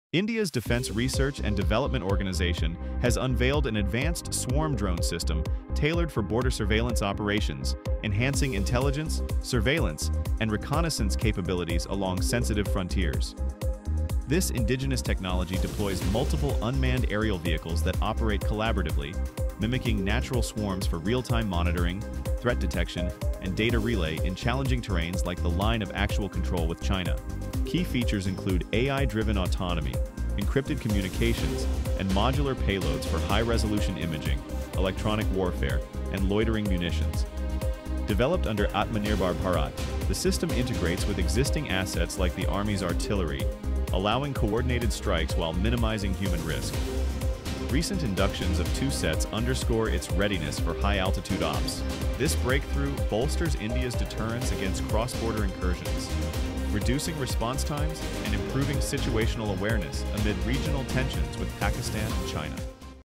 DRDO Develops Swarm Drone System for India Border Surveillance Ops
DRDO Develops Swarm Drone System for Border Surveillance Ops

India's DRDO has unveiled an advanced swarm drone system for enhanced border surveillance operations, capable of autonomous coordination among multiple UAVs for real-time monitoring, threat detection, and rapid response in challenging terrains. Integrated with AI for data analysis and anti-drone countermeasures, the system strengthens national security amid escalating regional tensions.